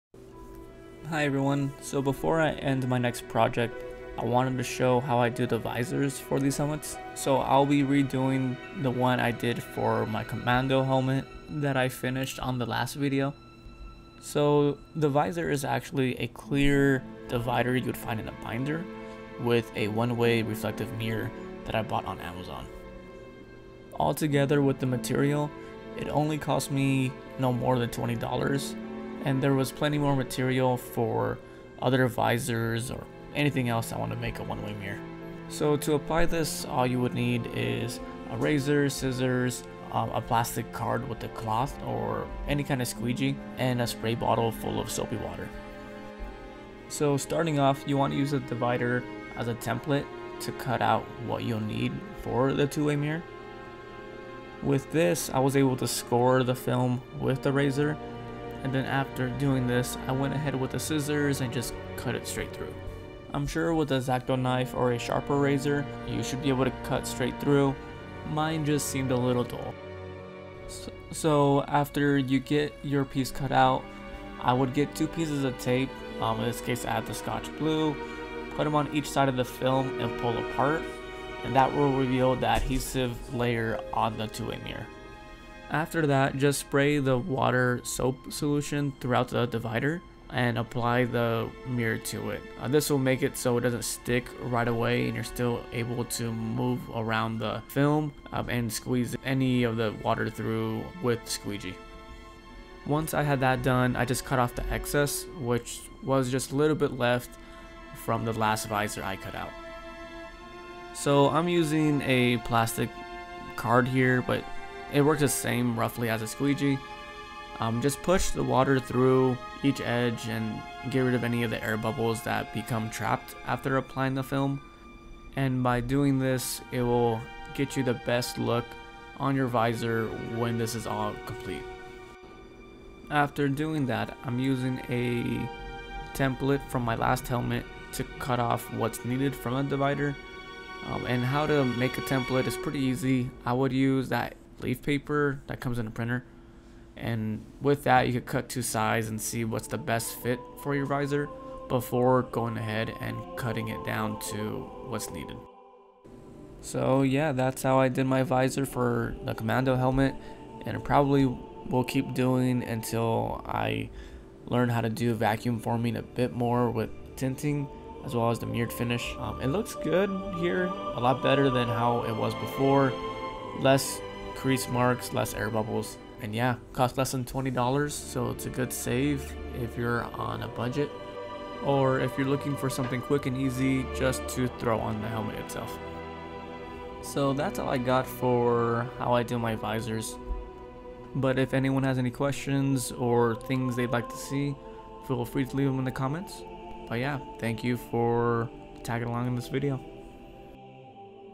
How to Make a Mirrored Visor Under $20
Hi everyone!
So as I continue to work on my EOD Helmet, I thought I'd make a short video on how I create my visors.

One Way Mirror Window Film Gold Silver

Here is the link to the shop I purchased the model

3D Printers I use - CR-10 V3
Print Settings
-0.6mm nozzle
-3 layer walls
-15% density
-7% support density
-75 mm/s speed

-Milwaukee M12 Detail Sander
-Filler Primer (Similar to the one I used in video)
-3M Sanding Blocks
-Precision Kit
-Cutting Pliers
-RIDGID Detail Vacuum Set
-Compressed Air Cans ( Air compressor works the same)

Sony A7SIII
Sigma 24-70mm
Go Pro Hero 10